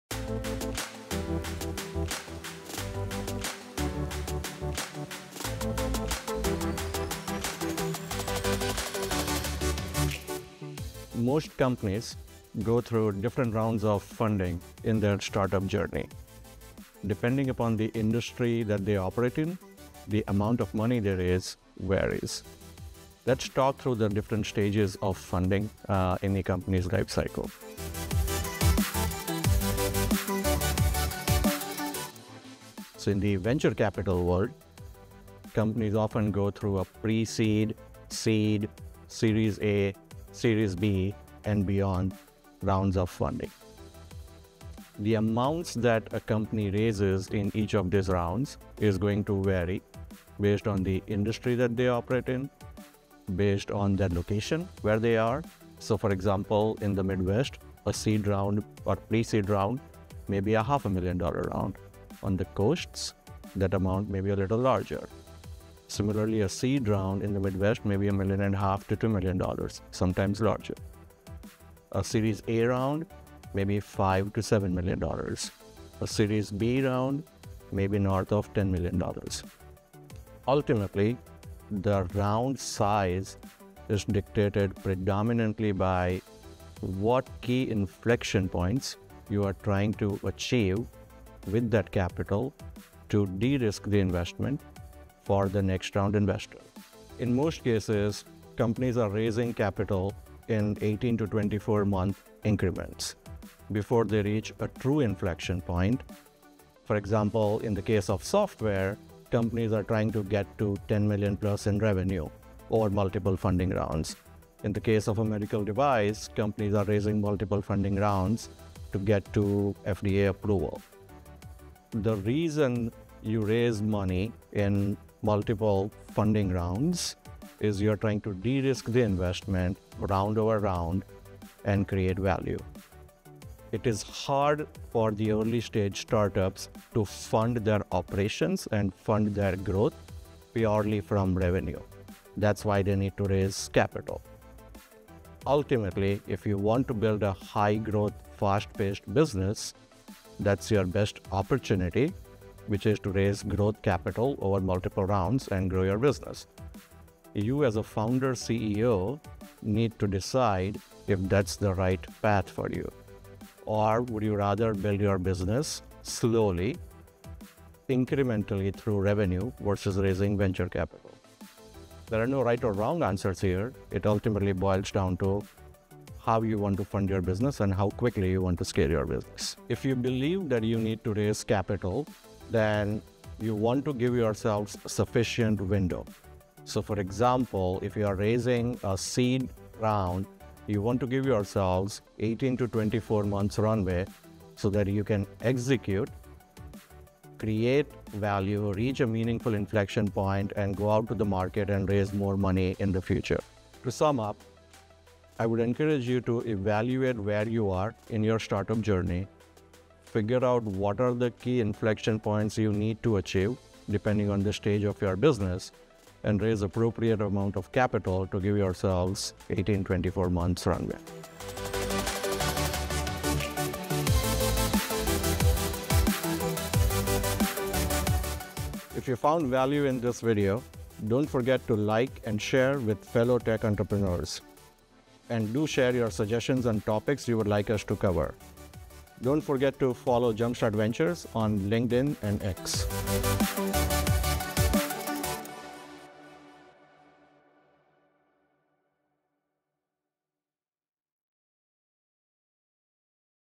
Understanding Funding Stages
Most companies go through several funding rounds in their journey.  Different business models and industries require different capital structures. In this episode of Demystifying Investing, Hardik talks about these stages and how you can navigate them effectively.

🌱 Key Insights Include: 🌱
✨ Pre-Seed Funding: Often the initial capital to kickstart your venture, with amounts typically around $500k.
💡 Seed and Seed Plus: The next phase to further validate and build your business, with funding around $2M-$3M.
🚀 Series A: A crucial stage for scaling with $5M-$7M raised to achieve product-market fit and other significant milestones.
📈 Series B and Beyond: Advanced funding rounds starting from $10M to propel substantial growth and market expansion.

Each funding stage is designed to support your company's growth and prepare it for the next level.

Raising capital is not just about the funds but also about reaching crucial business milestones and preparing for future growth stages.

An 18-month runway is recommended at each stage to ensure adequate time for traction and preparing for subsequent funding rounds.

📝 Takeaway: 📝
Evaluate your startup's current position and strategize your capital-raising journey thoughtfully, aligning your funding stages with your growth objectives and industry requirements.